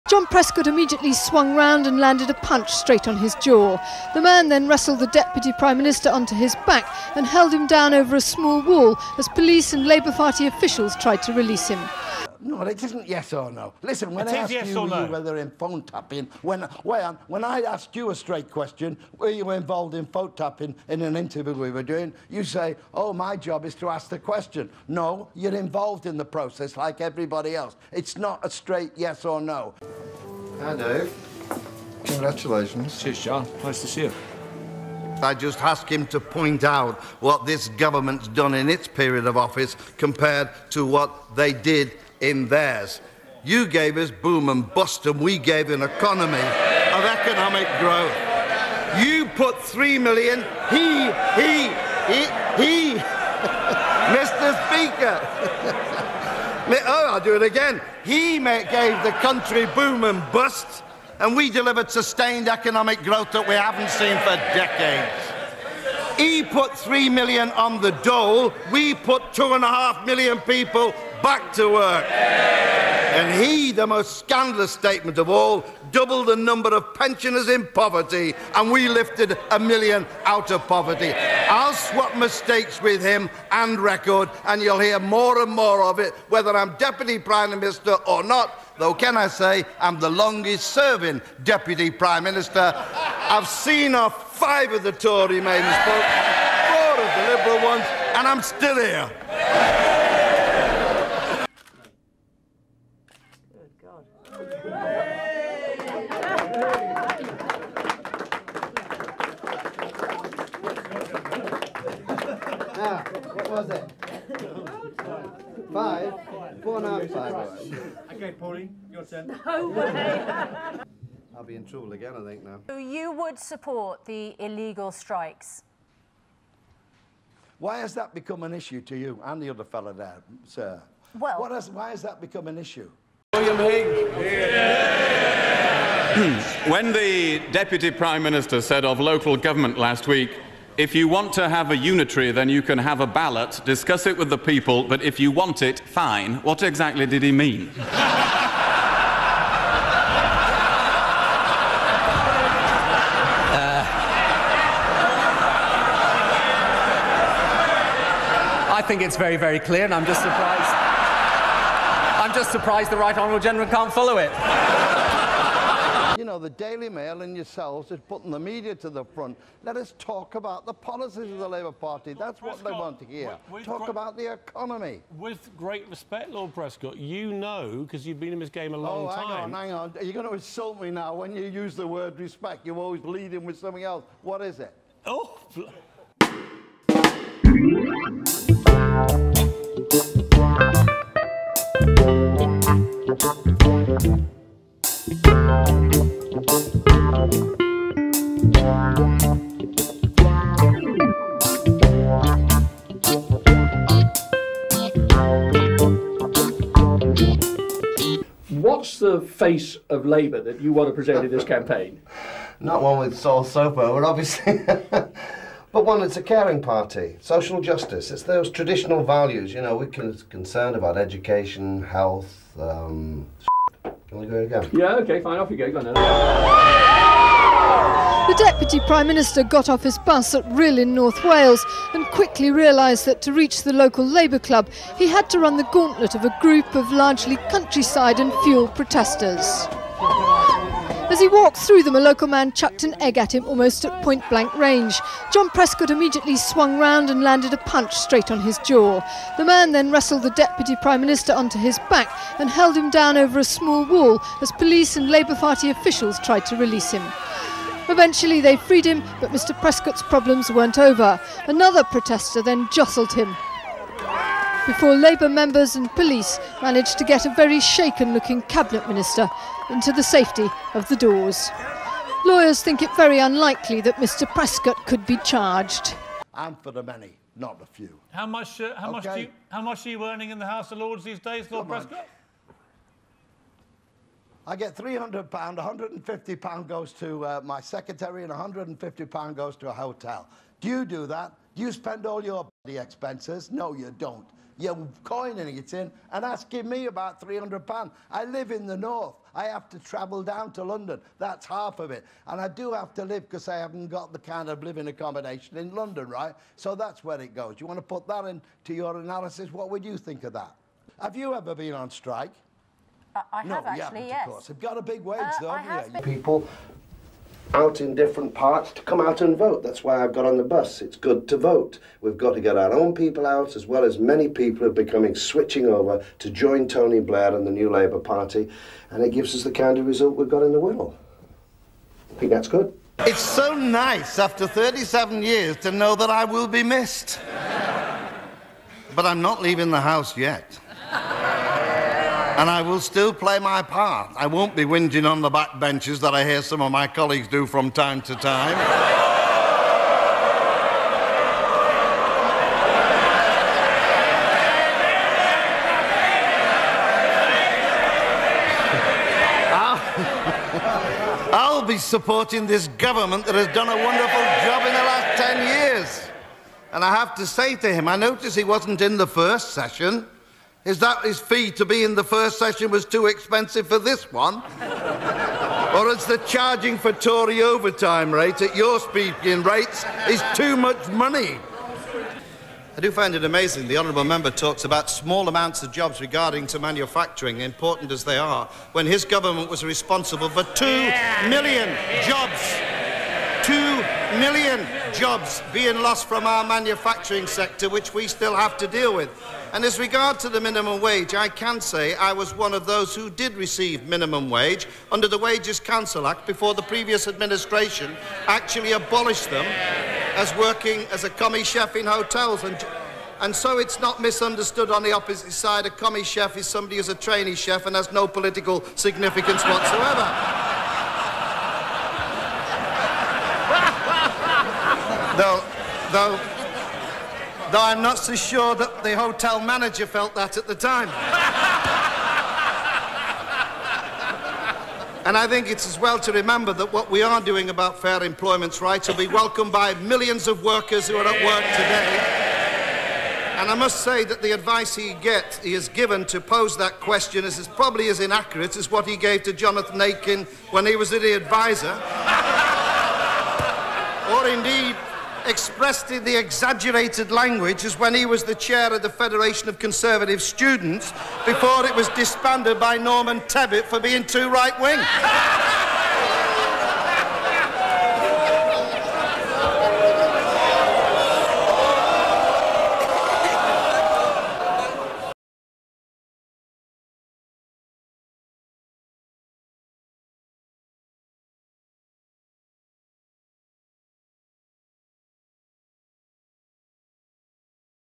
Just John Prescott's iconic moments
Titan of British politics John Prescott has died at 86.

Here's the best bits from our hero of the working classes and the trade union movement.